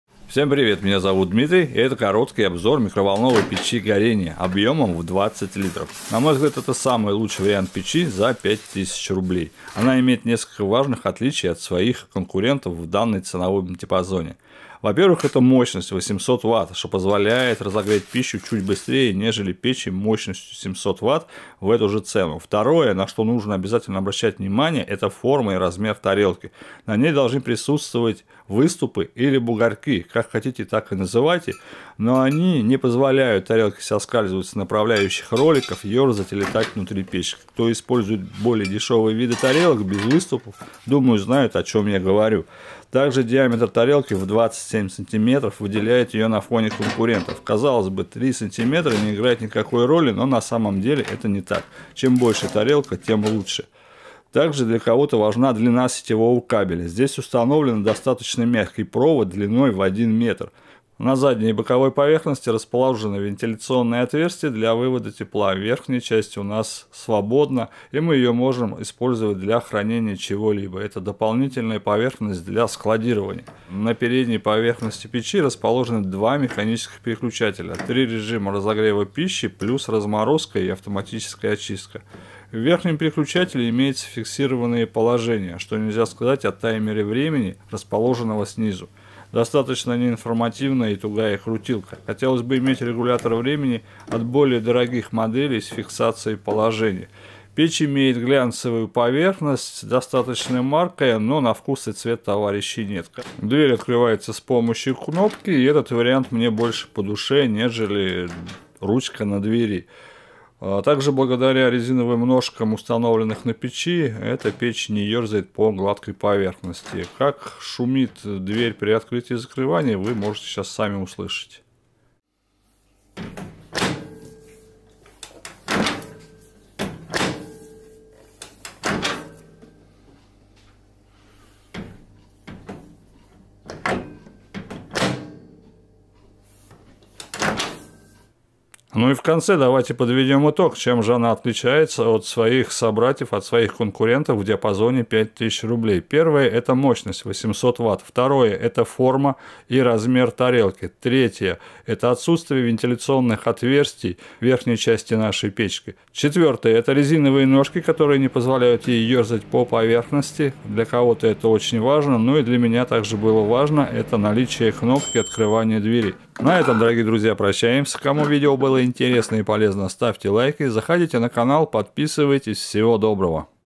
Лучшая Микроволновка до 5000 рублей. Обзор- Микроволновая печь Gorenje MO 20 Е .Как выбрать свч???
Микроволновая печь Gorenie MO20E1B с механическим управлением, мощностью 800 ватт , за пять тысяч рублей . Рассмотрим основные отличия  от конкурентов и плюсы данной микроволновой печи  Gorenie  MO20 E 1B .